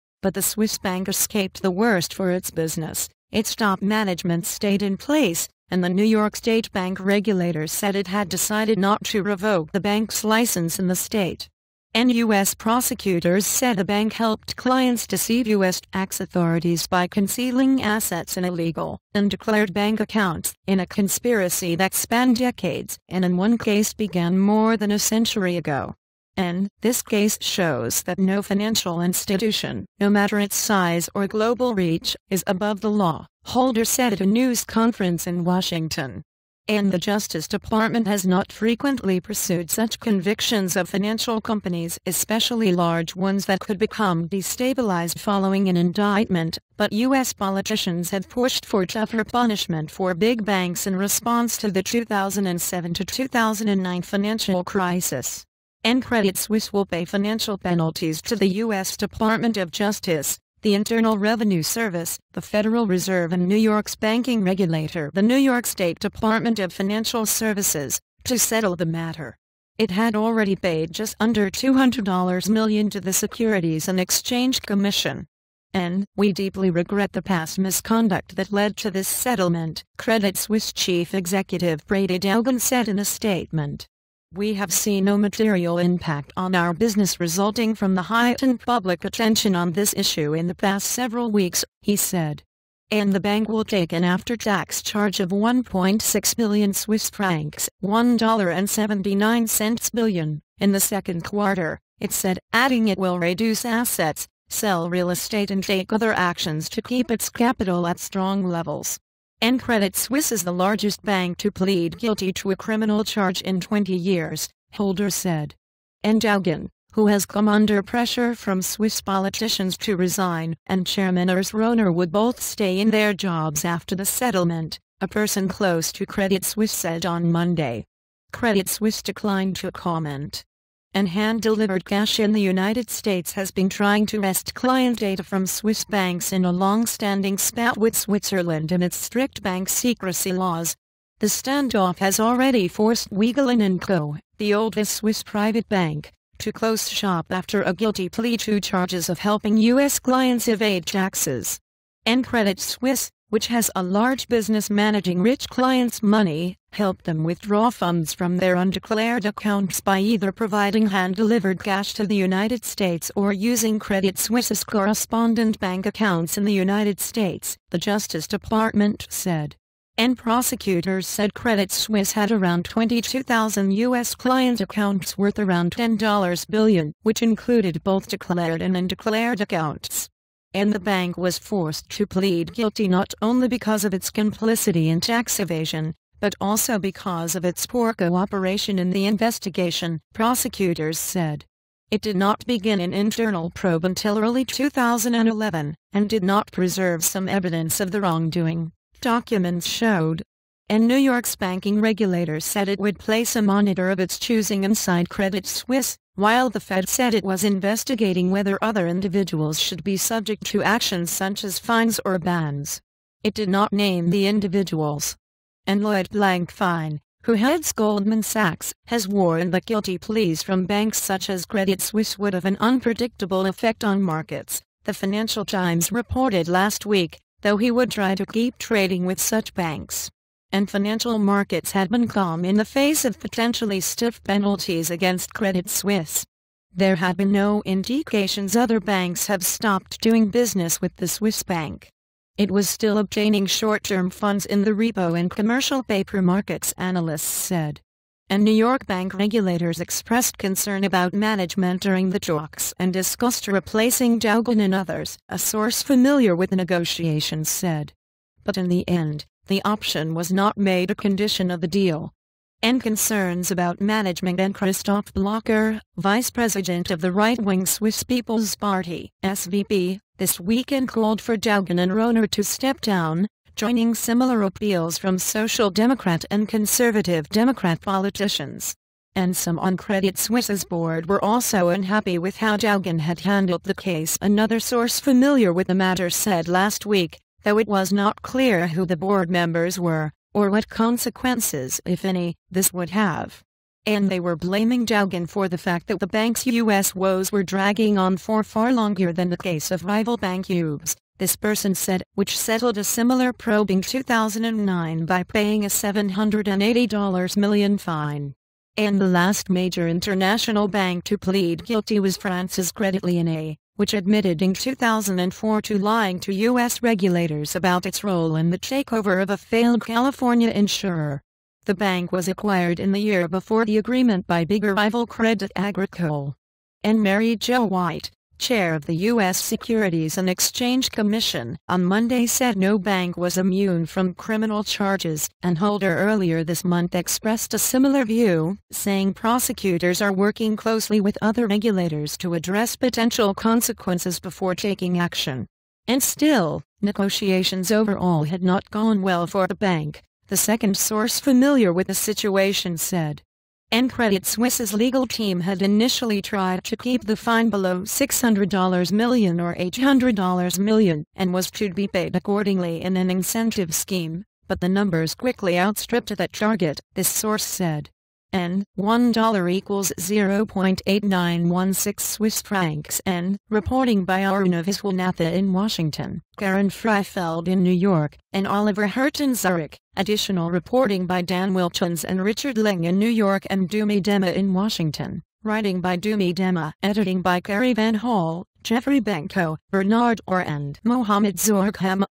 Credit Suisse escapes worst as it pleads guilty to U.S. charges
But the Swiss bank escaped the worst for its business - its top management stayed in place, and the New York state bank regulator said it had decided not to revoke the bank's license in the state. \n U.S. prosecutors said the bank helped clients deceive U.S. tax authorities by concealing assets in illegal, undeclared bank accounts, in a conspiracy that spanned decades, and in one case began more than a century ago. \n "This case shows that no financial institution, no matter its size or global reach, is above the law," Holder said at a news conference in Washington. \n The Justice Department has not frequently pursued such convictions of financial companies, especially large ones that could become destabilized following an indictment, but U.S. politicians have pushed for tougher punishment for big banks in response to the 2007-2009 financial crisis. \n Credit Suisse will pay financial penalties to the U.S. Department of Justice, the Internal Revenue Service, the Federal Reserve and Ne